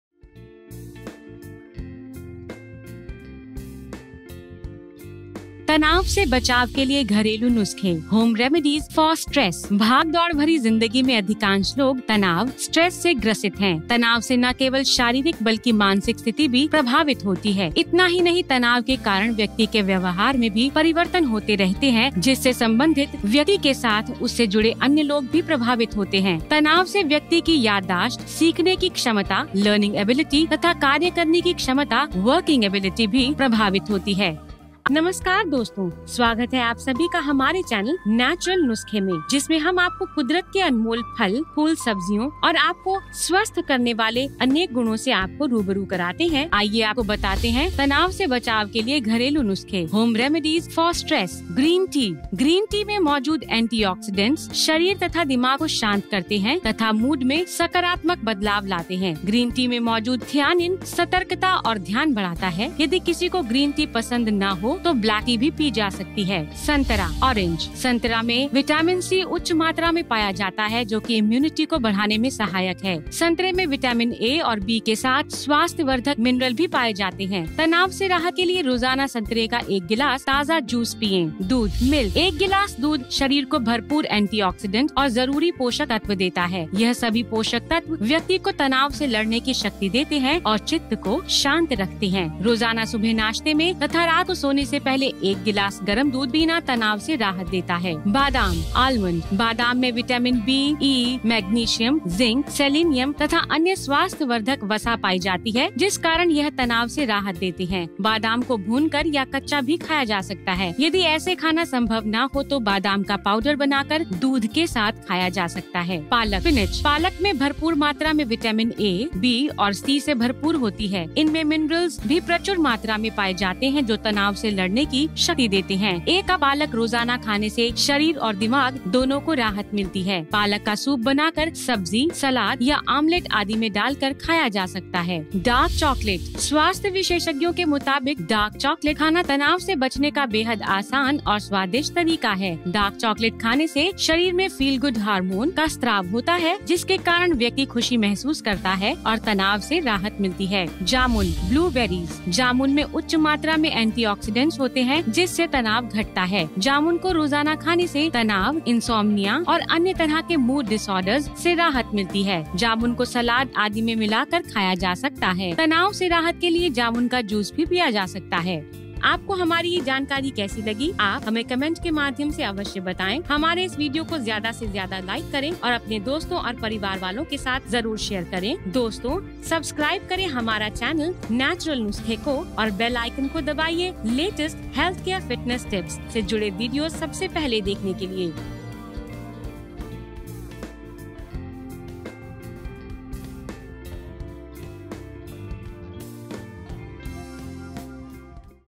तनाव से बचाव के लिए घरेलू नुस्खे || Home Remedies For Stress
भागदौड़ भरी दिनचर्या में अधिकांश लोग, तनाव (Stress) से ग्रसित हैं। तनाव से न केवल शारीरिक बल्कि मानसिक स्थिति भी प्रभावित होती है। इतना ही नहीं तनाव के कारण व्यक्ति के व्यवहार में भी परिवर्तन होते रहते हैं जिससे संबंधित व्यक्ति के साथ उससे जुड़े अन्य लोग भी प्रभावित होते हैं। तनाव से व्यक्ति की याददाश्त, सीखने की क्षमता (Learning ability), तथा कार्य करने की क्षमता (Working ability) भी प्रभावित होती है।
Most people are stricken with stress in a race-filled routine. Stress not only affects physical but mental conditions too. Not only this, there are changes in the behavior of the person due to stress, which affects the other person related to the person related to him. Stress affects the person's memory, learning ability, and working ability.